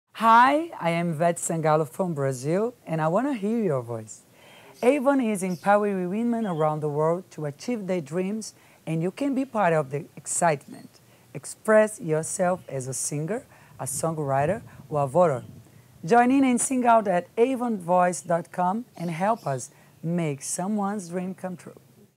IVETE SANGALO Explains Avon Voices (English)
Avon's first-ever global online Singing Talent Search for women, and Songwriting Competition for women and men, can turn dreams into reality.

To celebrate 125 years of empowering women and families throughout the world, Avon invites people around the globe to share their voices, join in, and sing out.

Sing your way to the top, write an original song, or help discover the next global singing sensation.